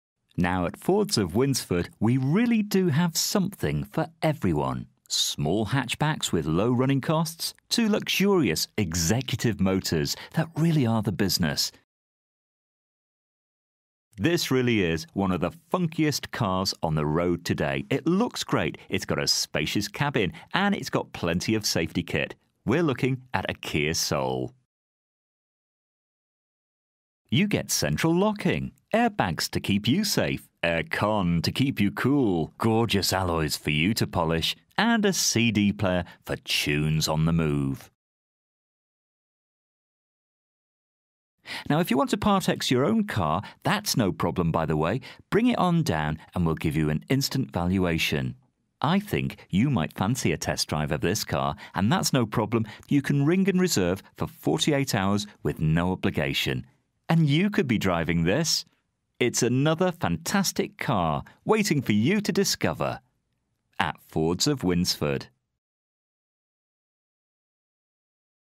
KIA SOUL DIESEL HATCHBACK (2010) 1.6 CRDI 2 5DR AUTO - LT10RXD
DETAILS:
First registered: 27/03/2010
Current mileage: 38460
Registration: LT10RXD
Colour: BLACK

FEATURES:
all round electric windows, remote central locking, cloth interior, all round airbags, air conditioning, pas, alloy wheels, solid paint, cd / mp3, parcel shelf presen

This used car is for sale at Fords of Winsford Car Supermarket in Cheshire, UK